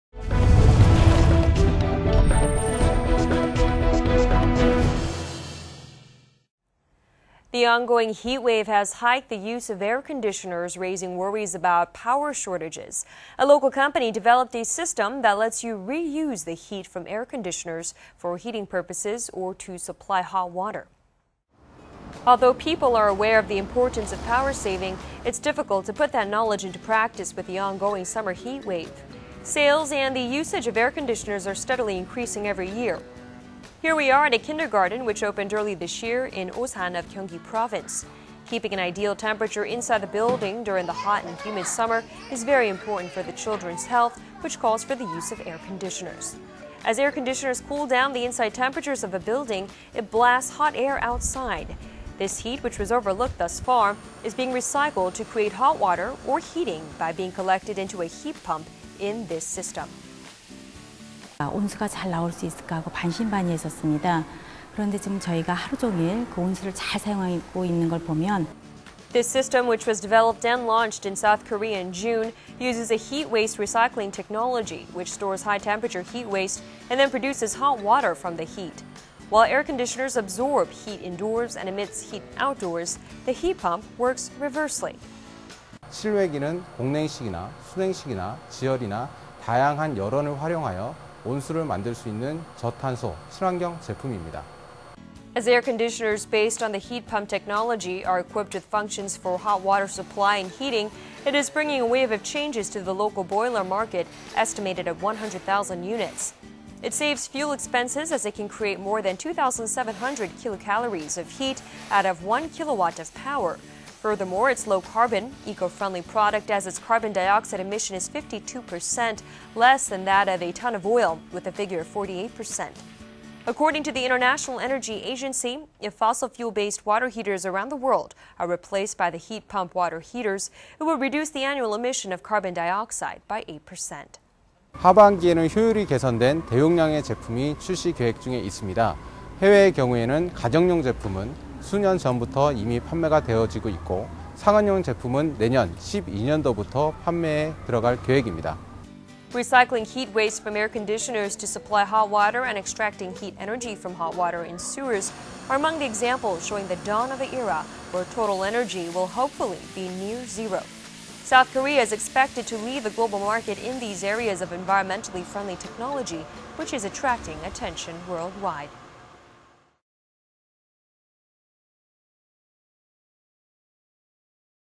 [Industry professional news channel itsTV] Supplying hot water with heat from air conditioners
[anchor]

 The on-going heat wave has hiked the use of air conditioners, raising worries about power shortages. A local company developed a system that lets you reuse the heat from air conditioners for heating purposes or to supply hot water.

[report]

 Although people are aware of the importance of power saving, it is difficult to put that knowledge into practice with the on-going summer heat wave. Sales and the usage of air conditioners are steadily increasing every year.

 Here we are at a kindergarten which opened early this year in Osan of Gyeonggi province. Keeping an ideal temperature inside the building during the hot and humid summer is very important for the children's health, which calls for the use of air conditioners.

 As air conditioners cool down the inside temperatures of a building, it blasts hot air outside.

 This heat, which was overlooked thus far, is being recycled to create hot water or heating by being collected into a heat pump in this system.

         [Interview - Choi In-sil, principal, Segyo Kindergarten]

 Q. What are the pros and power-saving effect of the Hydro Kit system?

 ~ We were doubtful whether it could create hot water but such disbelief was dispelled as we can use hot waver all day now.

 This system, which was developed and launched in South Korea in June, uses a heat waste recycling technology, which stores high-temperature heat waste and then produces hot water from the heat.  While air conditioners absorb heat indoors and emits heat outdoors, the heat pump works reversely.

   [Interview - Kang Yoon-tae, Manager, LG Electronics CAC Engineering Team]

 Q. How does the Hydro Kit Heat Pump work?

 ~ The air conditioner's outdoor unit is a environmentally-friendly, low carbon product that creates hot water through various sources of heat and by various ways, such as air cooling or water cooling.

 As air conditioners based on the heat-pump technology are equipped with functions for hot water supply and heating, it is bringing a wave of changes to the local boiler market estimated at 100,000 units.

It saves fuel expenses as it can create more than 2,700 kilocalories of heat out of 1kilowatt of power. Furthermore, it is low-carbon, eco-friendly product as its carbon dioxide emission is 52%,  less than that of a  ton of oil with a figure of 48 percent.

 According to the International Energy Agency, if fossil-fuel-based water heaters around the world are replaced by the heat-pump water heaters, it will reduce the annual emission of carbon dioxide by 8 percent.

 Q. What are the future plans for strategies and overseas launches?

 ~ During the rest of the year, we plan to launch a highly efficient model that enhances productivity. For the overseas market, models for household use have been on sale for several years and commercial products will go on sale next year.

 Recycling heat waste from air conditioners to supply hot water and extracting heat energy from hot water in sewers are among the examples showing the dawn of a era where total energy will hopefully be 'near zero.' South Korea is expected to lead the global market in these areas of environmentally friendly technology, which is attracting attention worldwide.